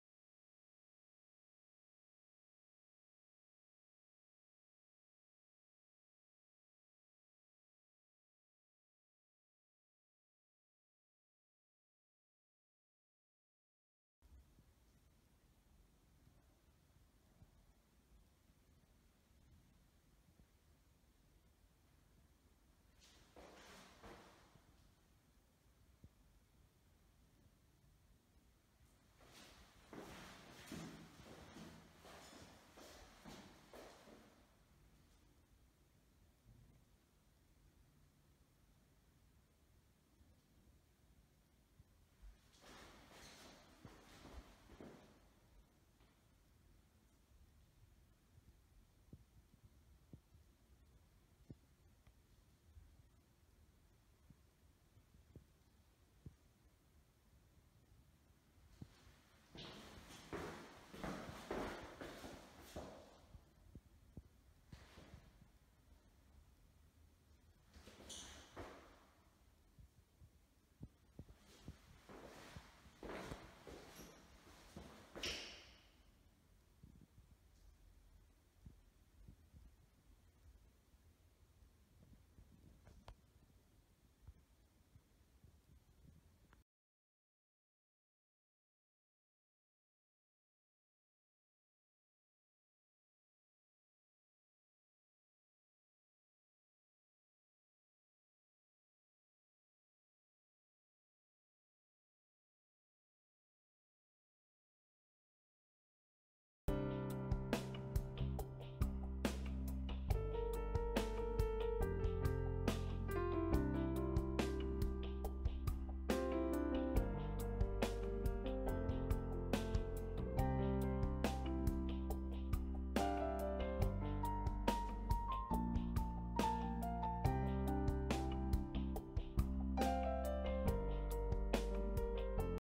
Esmalte Afinado Valacryl Color Azul Grisacio
Terminación.
Habitación pintada con esmalte al agua de Valentine.